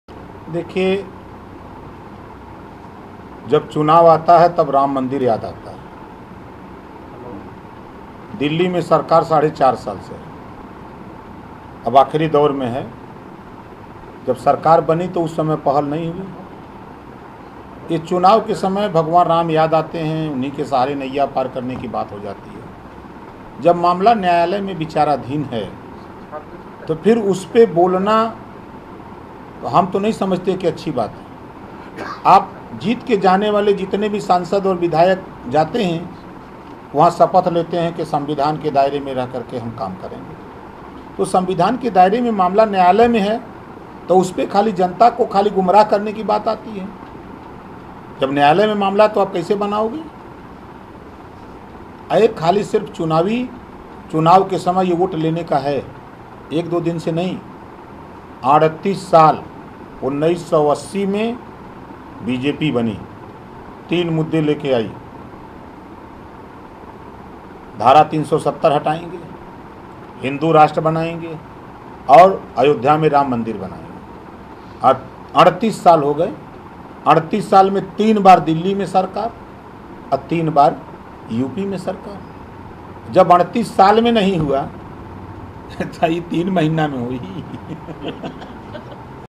ओमप्रकाश राजभर बोले - जब चुनाव आता है, तब बीजेपी को राम मंदिर याद आता है
हरदोई : उत्तर प्रदेश में योगी सरकार के कैबिनेट मंत्री ओमप्रकाश राजभर अपने विवादित के चलते चर्चा का विषय बने रहते हैं. गुरुवार रात मुरादाबाद जाते समय हरदोई में पत्रकार वार्ता के दौरान उन्होंने बीजेपी सरकार पर तंज कसते हुए कहा कि चुनाव के समय ही भगवान राम याद आते हैं. सरदार पटेल की मूर्ति को लेकर भी उन्होंने तंज कसा कहा कि ये फिजूलखर्ची है.